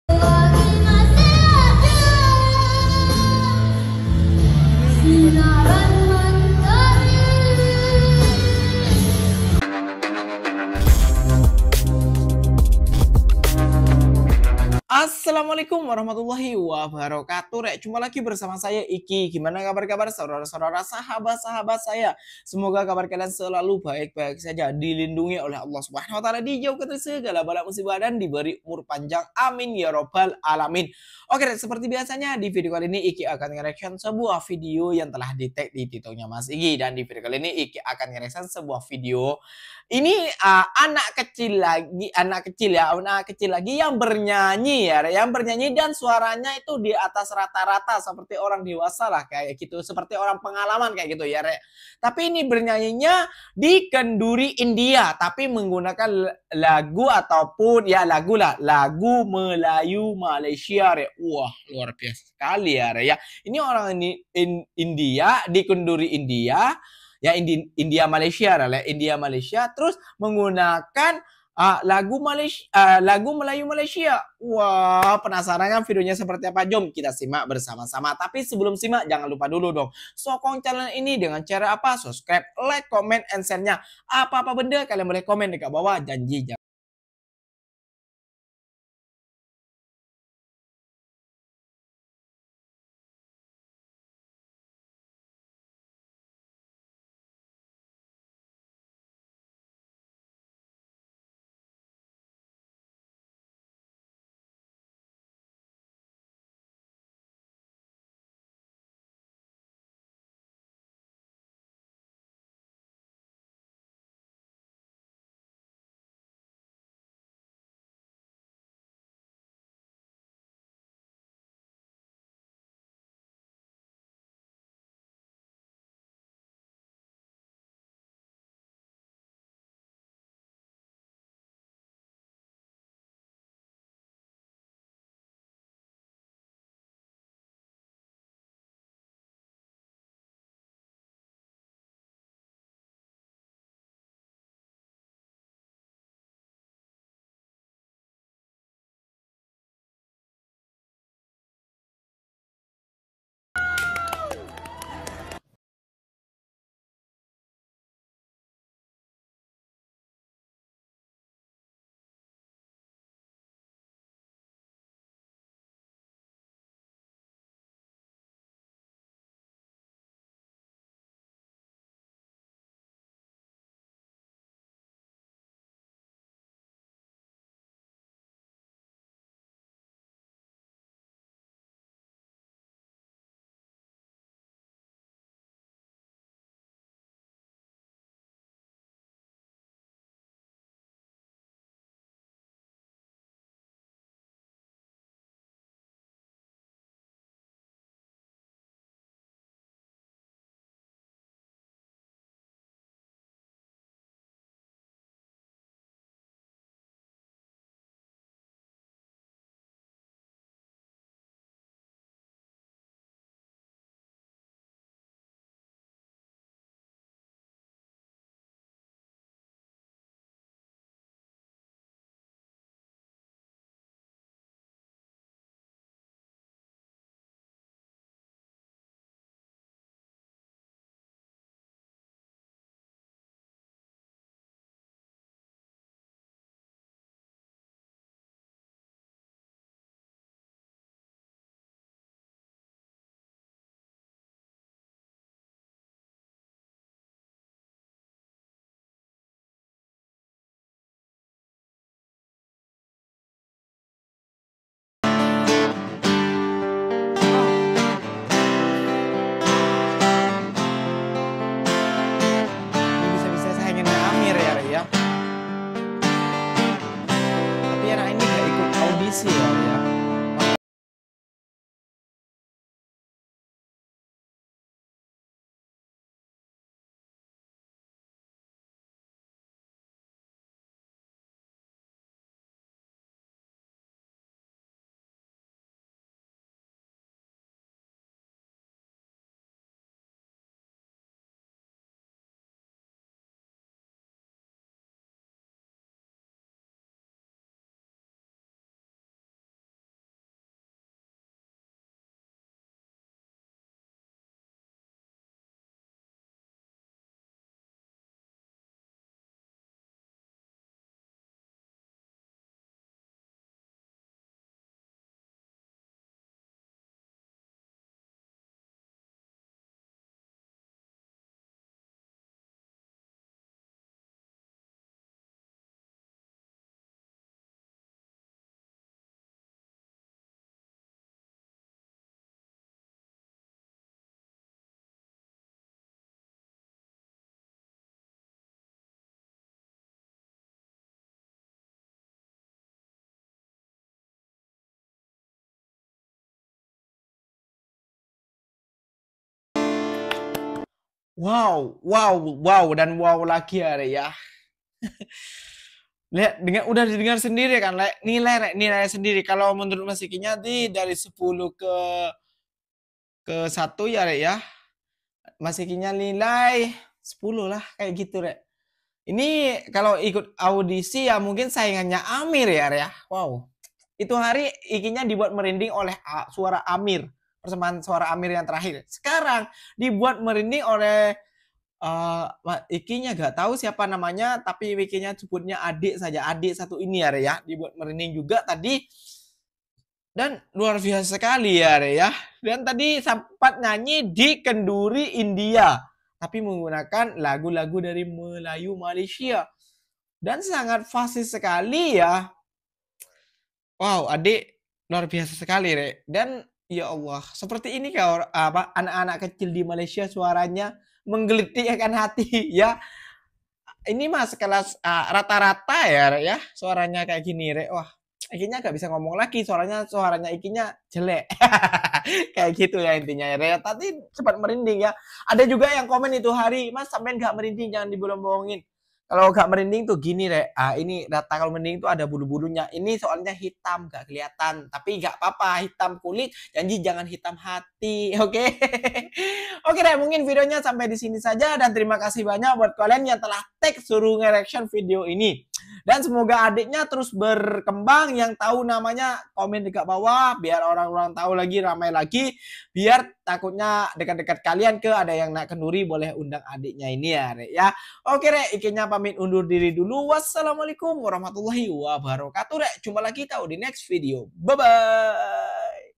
WOW MALAYSIA BANYAK MEMILIKI ANAK KECIL YANG BERSUARA EMAS
Assalamualaikum rek jumpa lahi bersama saya iky yang suka reaction video menarik tentang Malaysia